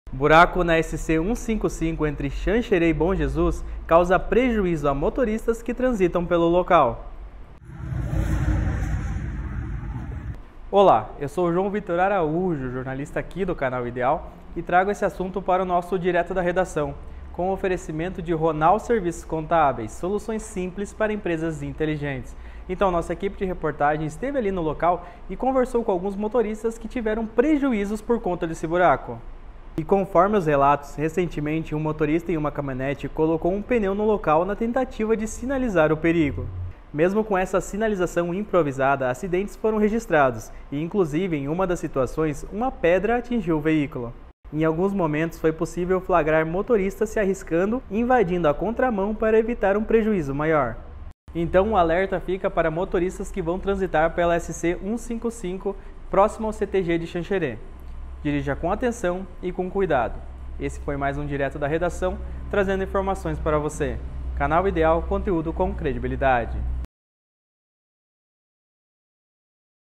Buraco na SC-155 entre Xanxerê e Bom Jesus causa prejuízos e coloca motoristas em risco
Na SC-155 entre o município de Xanxerê e Bom Jesus, um buraco tem causado prejuízos e colocando motoristas em risco.

A equipe de reportagem do Canal Ideal esteve no local e conversou com motoristas que foram prejudicados. Inclusive um deles foi atingido por uma pedra.